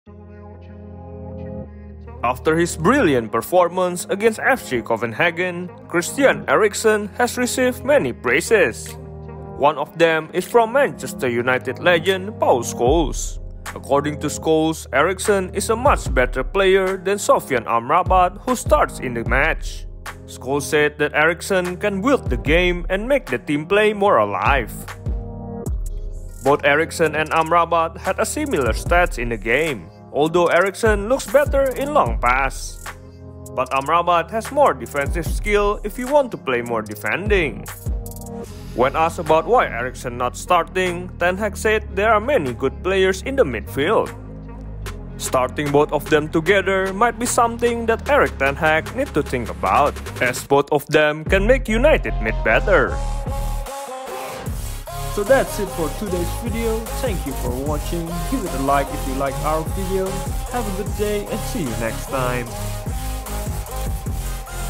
Paul Scholes Believes Christian Eriksen A Much Better Player Than Sofyan Amrabat
Paul Scholes believes that Christian Eriksen should start rather than Sofyan Amrabat.